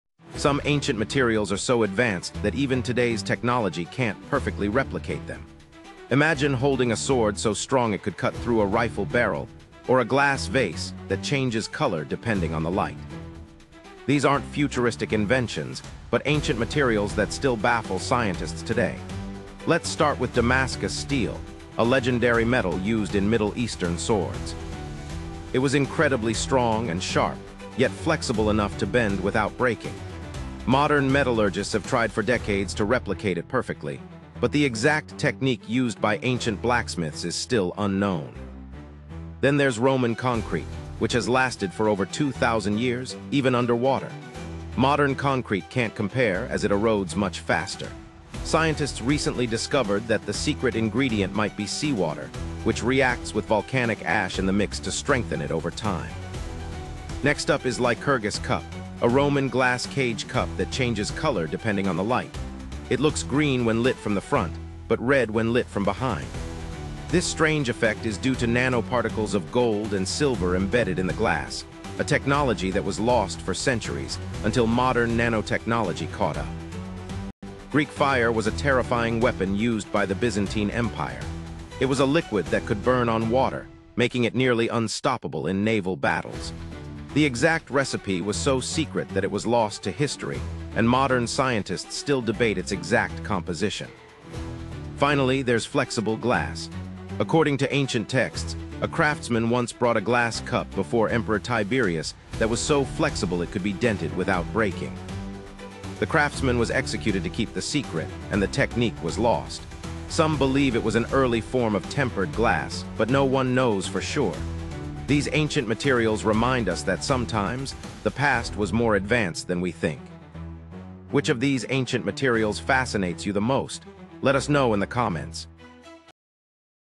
Ancient Materials Modern Science Still Can’t Copy
From swords to cement, these ancient materials still baffle modern scientists.